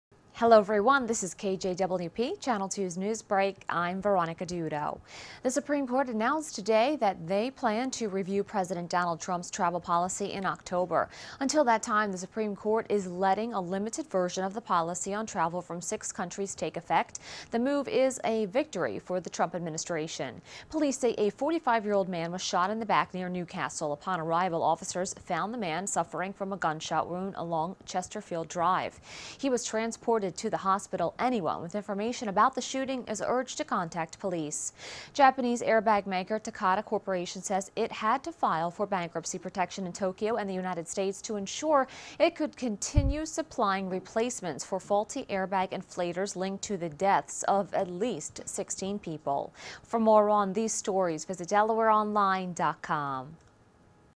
KJWP Daily NewsBreaks 12:44PM - 6.26.17
SCOTUS to review the Travel Ban; Man shot near New Castle; Takata files bankruptcy